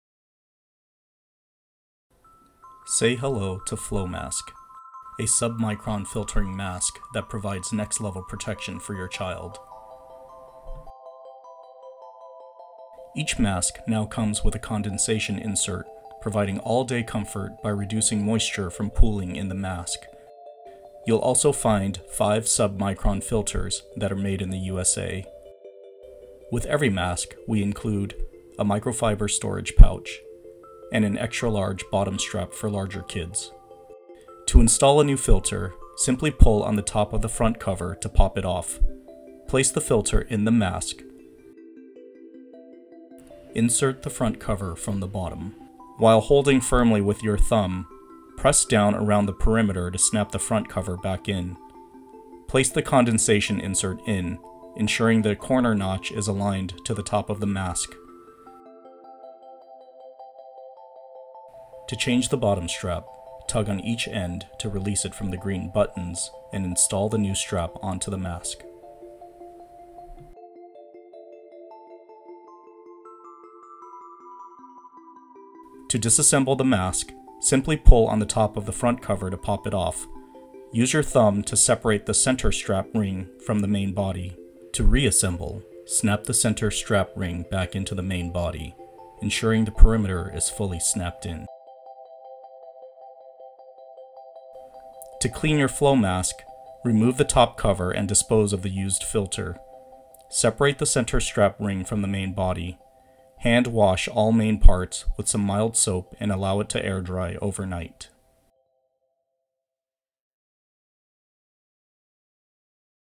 Flo Mask - How To Video
Quick tutorial on getting you started with your new Flo Mask.